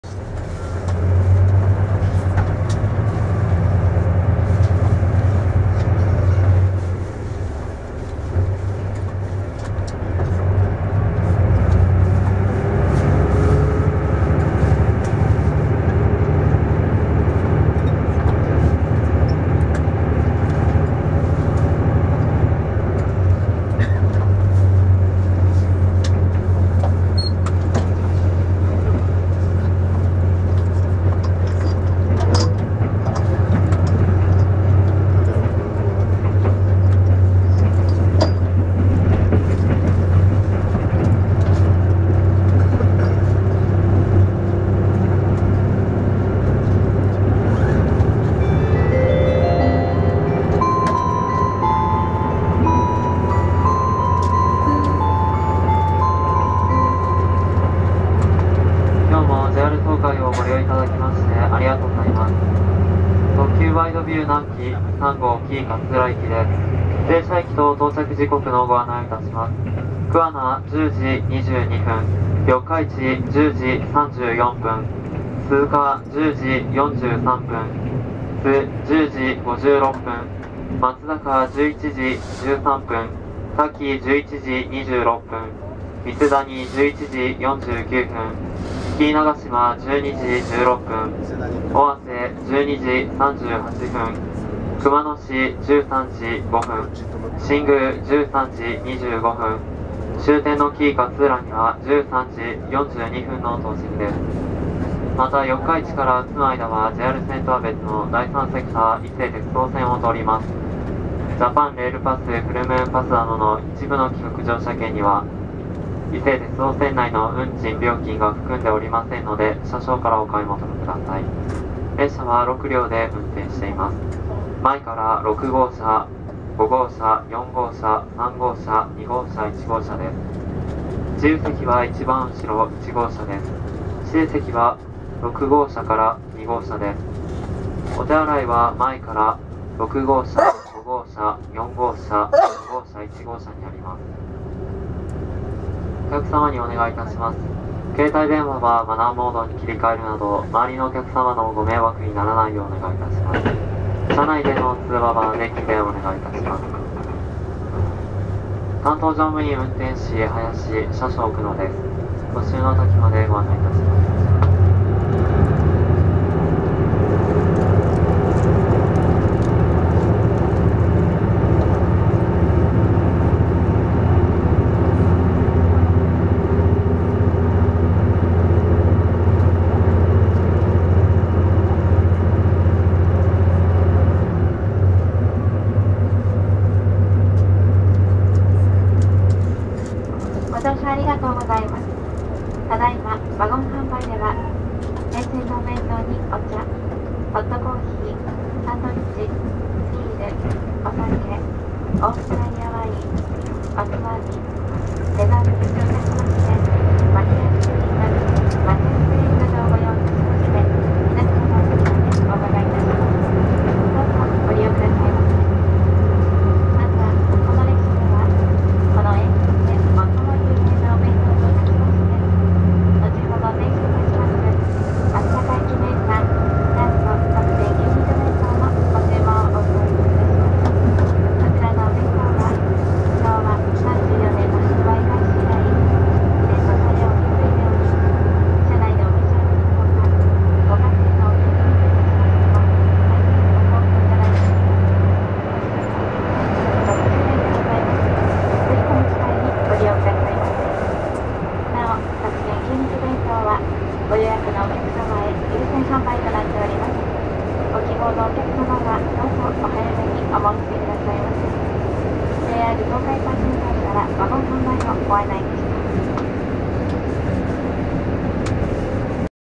特急ワイドビュー南紀 車窓風景 名古屋-八田 キハ85
JR東海特急ワイドビュー南紀 名古屋出発から次の駅付近までの車窓風景です。